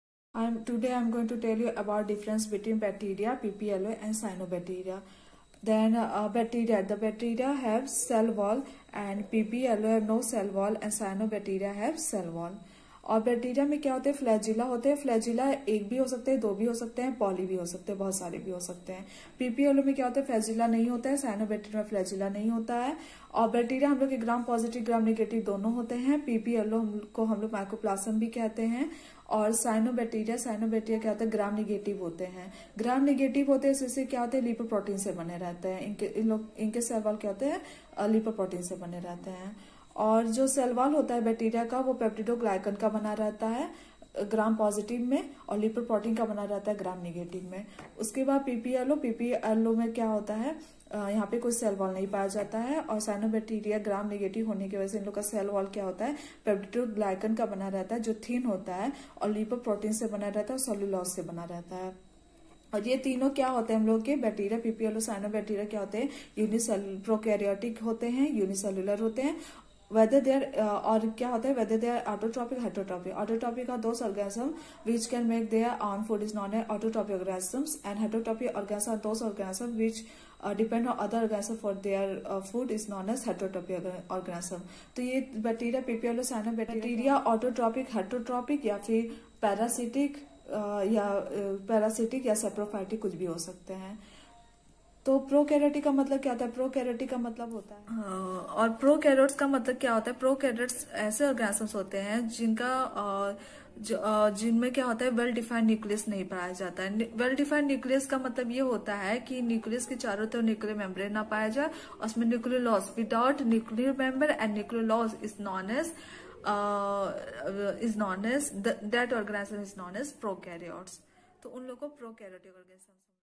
Monera - Kingdom i.e Bacteria,Cyamophyta,Mycoplasm 🌳🌱🦑🐬
Monera Kingdom i.e Bacteria, Cyanophyta & Mycoplasm.

In this video I have explained about Kingdom Monera and Chart so you can understand easily.